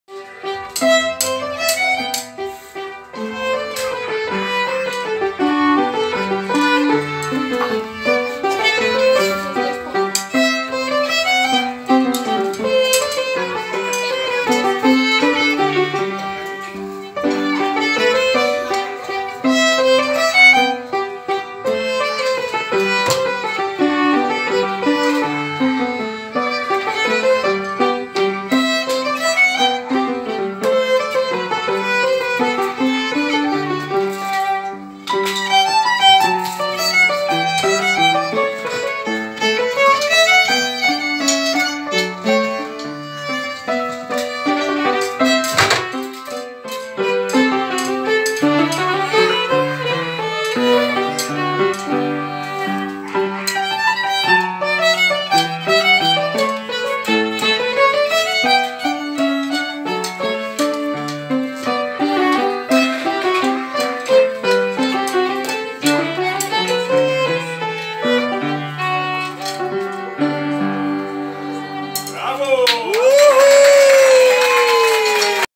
Show de Talentos Julho 2022
Famílias se encontram um domingo por mês para confraternizar e seus filhos mostram o que estão aprendendo nas aulas de musicalização. Aqui, Antoni e seu professor, Sales.🎶🎹🎻💙